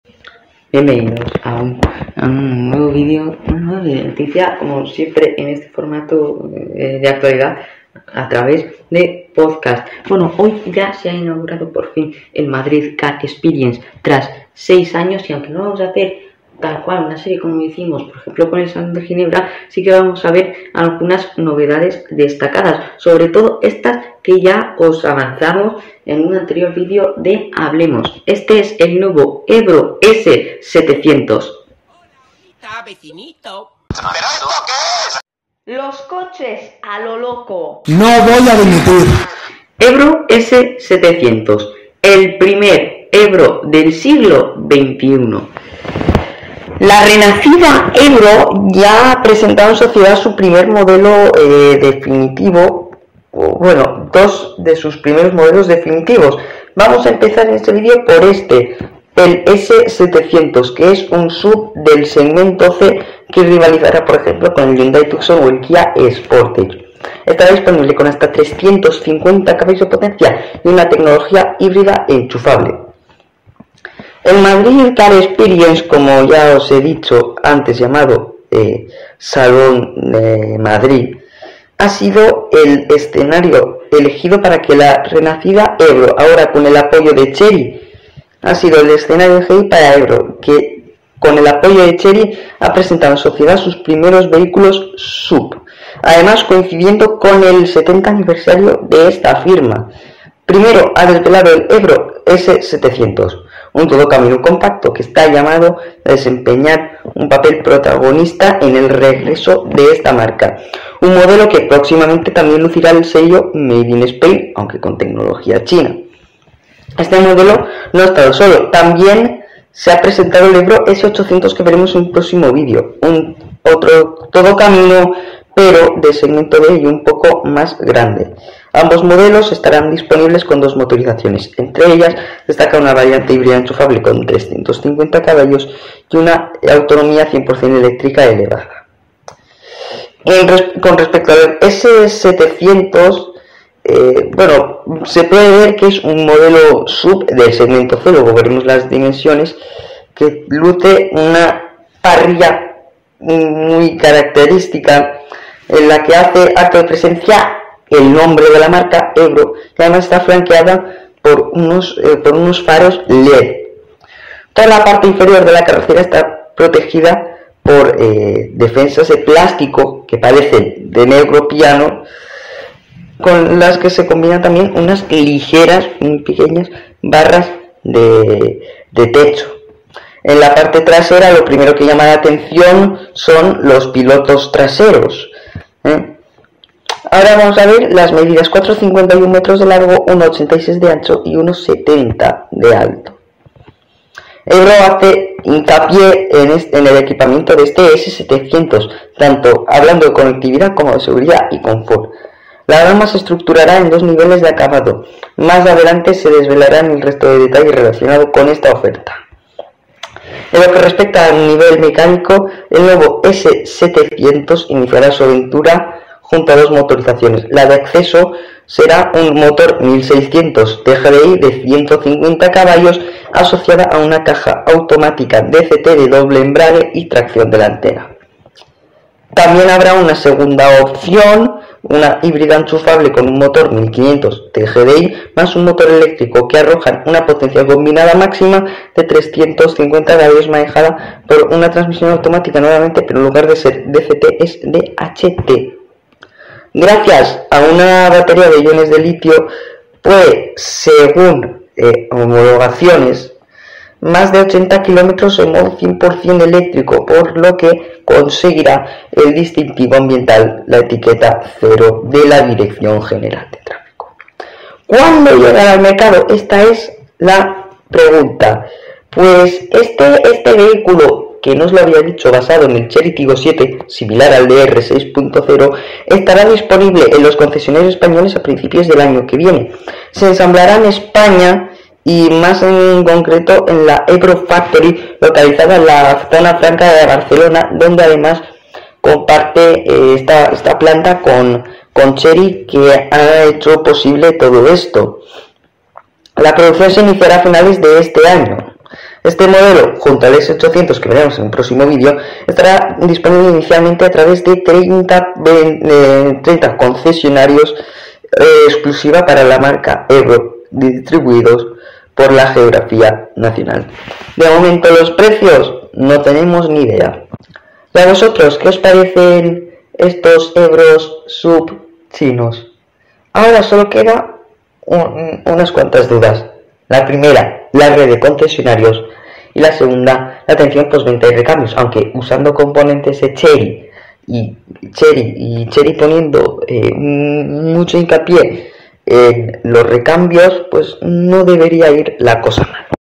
EBRO S700: El renacer chino de la histórica marca (Podcast)
Aprovechando que la renacida EBRO se ha aliado con Chery para producir en la zona Franca de Barcelona, han aprovechado el Madrid Car Experience (anteriormente llamado Salón del Automóvil de Madrid) para desvelar dos modelos nuevos para EBRO. El primero, llamado S700 es un SUV basado en el Chery Tiggo 7 Pro (vendido en España como DR 6.0), con un tamaño similar al Kia Sportage. Este vehículo estará disponible tanto con una utilización de gasolina como en una variante híbrida enchufable, y se fabricará a finales de este año para empezarse a venderse en el 2025 en España.